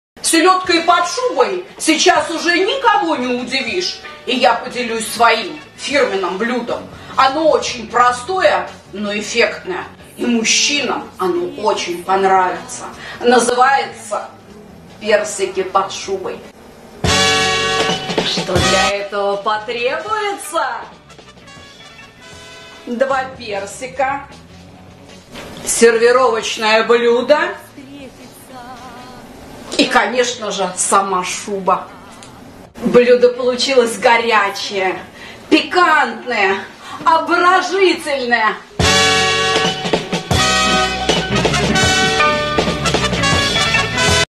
Секрет новогоднего салата персики под шубой
Потому что селёдкой под шубой давно никого не удивишь, а вот персиками под шубой удивить можно и даже нужно! Вы попробуйте, - вдруг понравится;) Причём, не только Вам!

Давайте, крошки, жду обратной связи - понравился мой рецепт или нет? И будете ли готовить?)😂😂😂